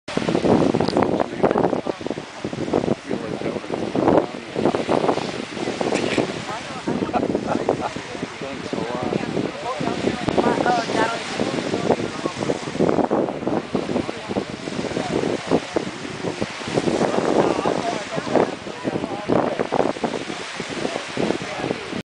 gillnet on humpbacks mouth.MPG
gillnet wrapped around animals mouth